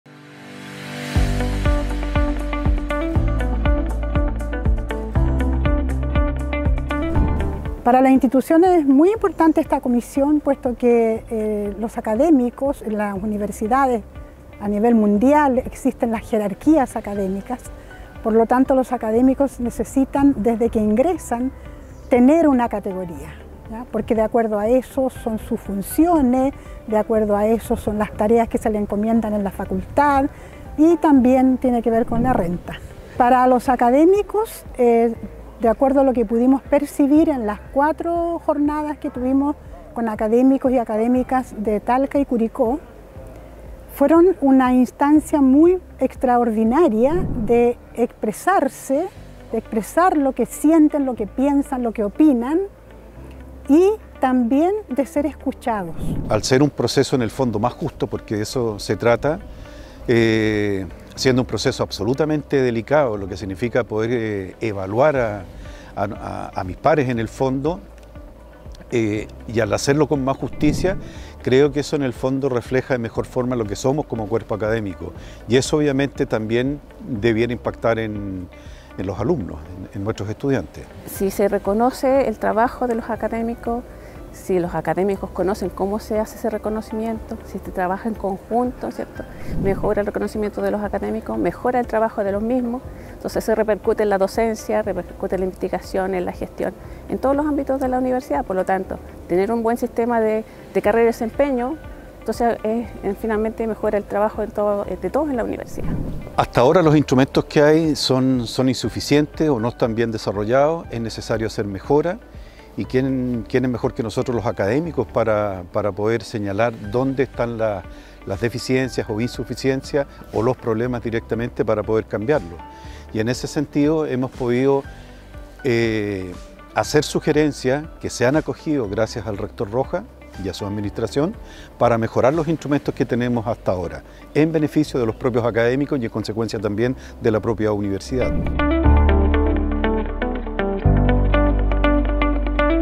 Comisión de Carrera y Desempeño Académico UCM
La Comisión de Carrera y Desempeño Académico recoge las impresiones de los académicos y académicas en las jornadas de trabajo coordinadas por la Vicerrectoría Académica, para reconocer el desarrollo académico y trabajar en la mejora de la Categorización y Promoción Académica.